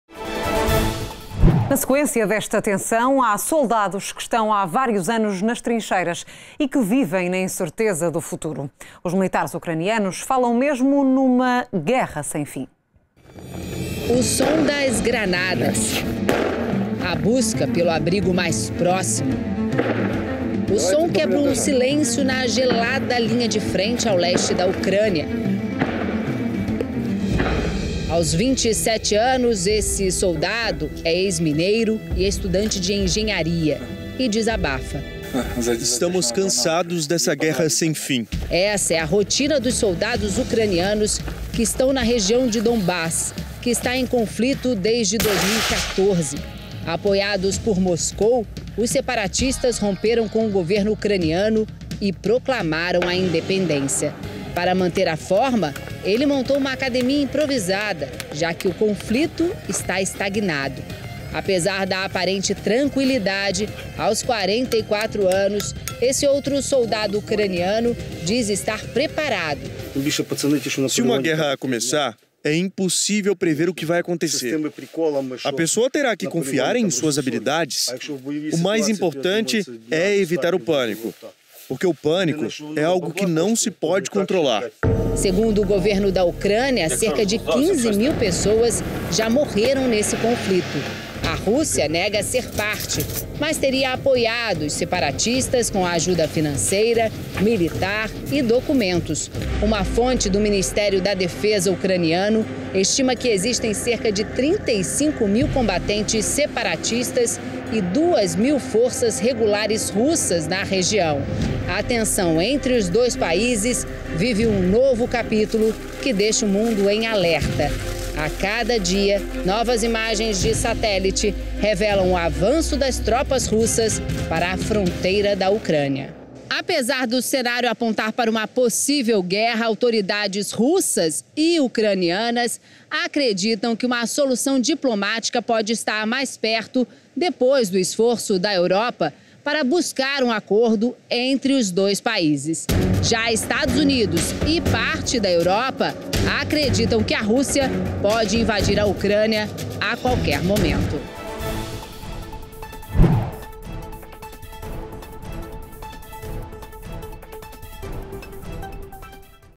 "Guerra sem fim"
Na sequência desta tensão, há soldados que estão há vários anos nas trincheiras e que vivem na incerteza do futuro. Os militares ucranianos falam mesmo numa guerra sem fim.